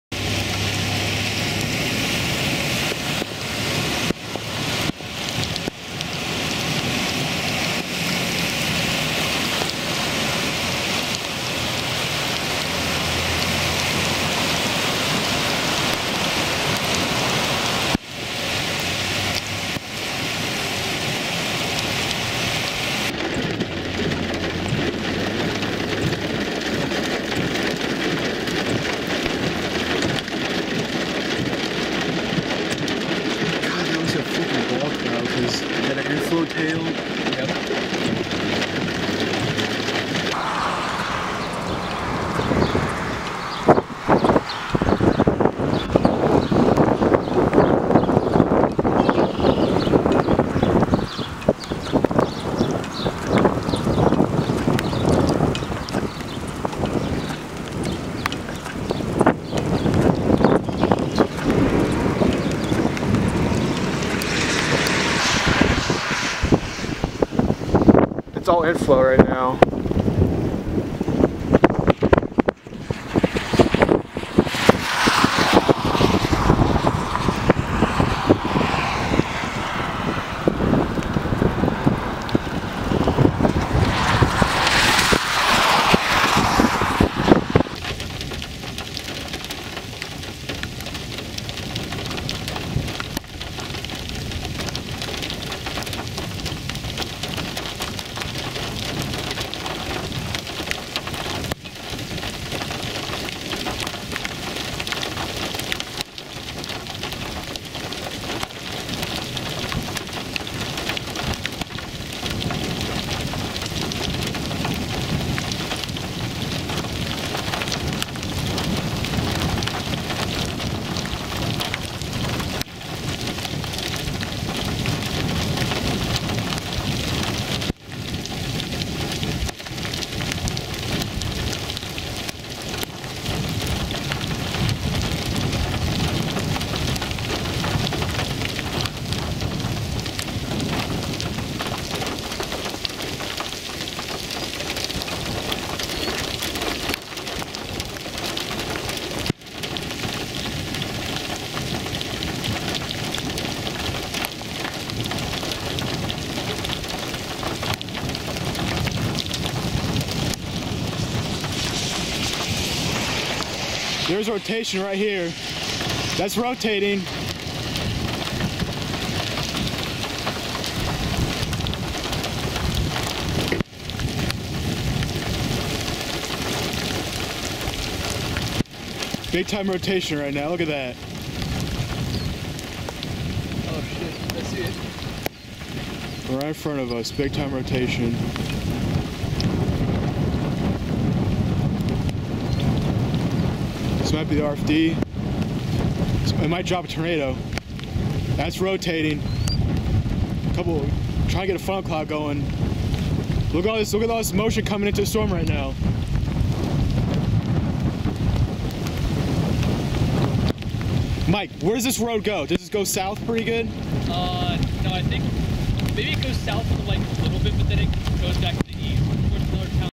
5/4/2015 Macomb, IL Flooding and Storm Structure/Rotation
Severe thunderstorms rolled across the Macomb, IL area late Monday afternoon into the early evening hours bringing flooding rain, hail,and even some rotation on the south side of the storm.